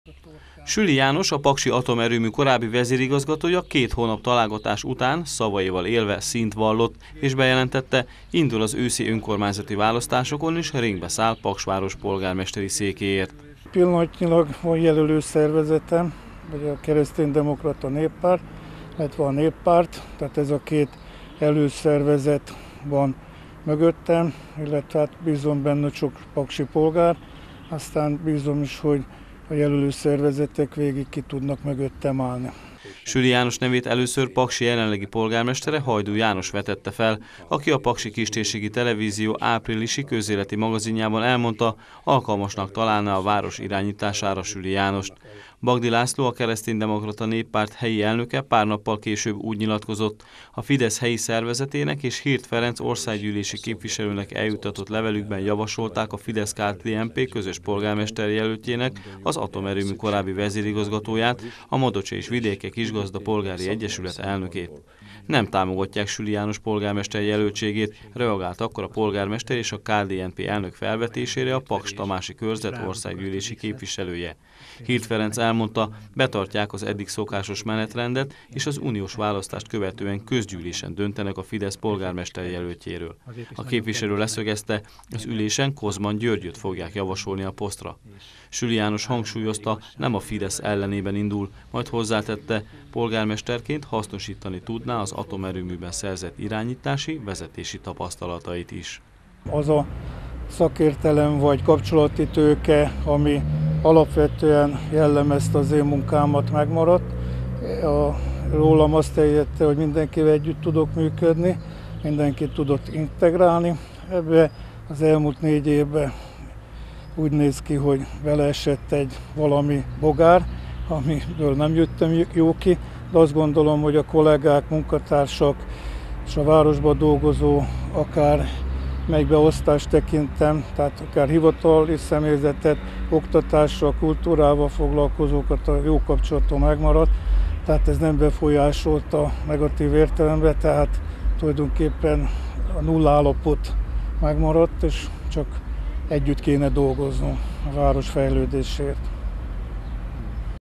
Polgármesterjelöltként indul Süli János
Bejelentette indulási szándékát az őszi önkormányzati választáson polgármester jelöltként Süli János. A paksi atomerőmű korábbi vezérigazgatóját jelenleg a Kereszténydemokrata Néppárt és a Néppárt támogatja.